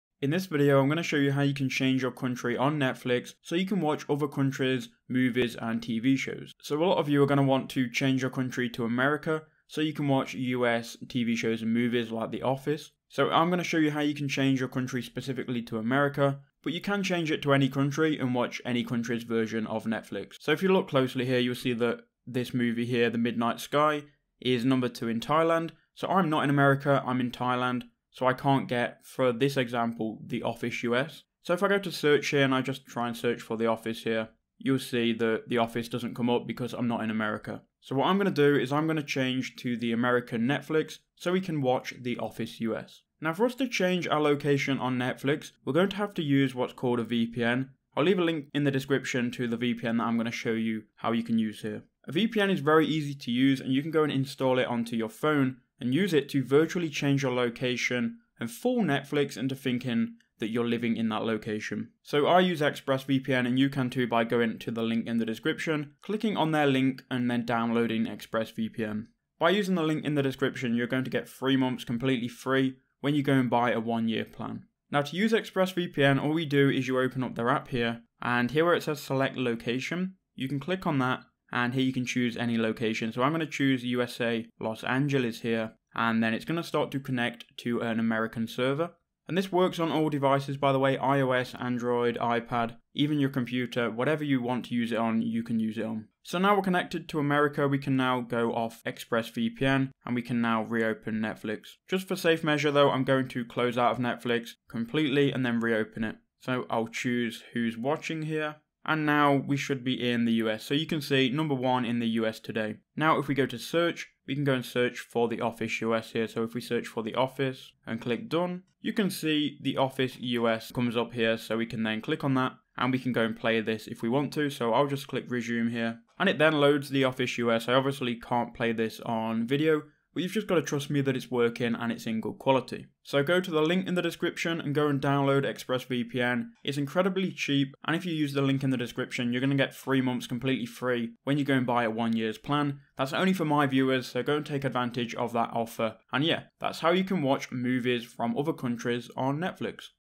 How To Change Country on Netflix on Phone (2021)
Chance Netflix country to American or any county to watch their selection of movies and tv shows! Did this help? Let me know below :)